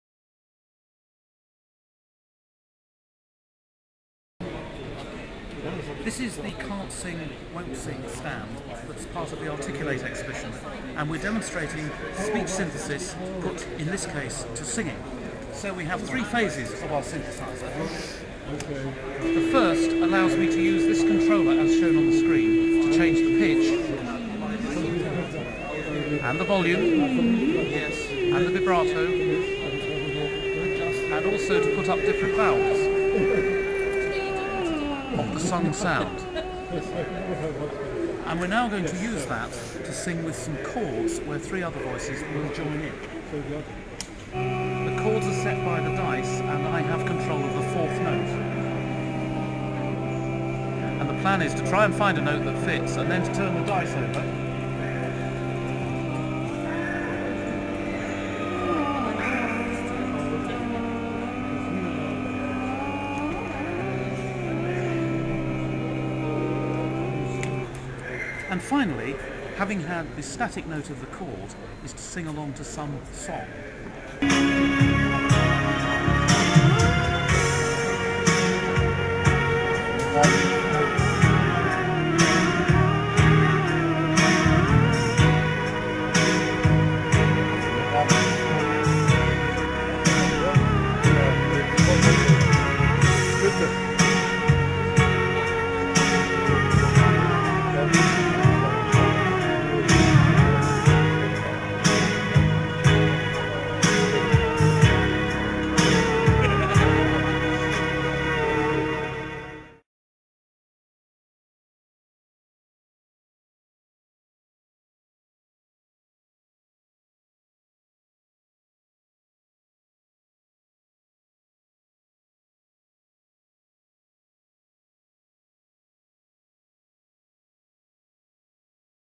"Can't sing won't sing"
"Can't sing won't sing" is a public display of electronic singing synthesis created as part of the UK EPSRC's (Engineering and Physical Sciences Research Council) CREST (Creative Speech Technology) network and on show in "Articulate" in Sheffield, UK, 4-6 December, 2012.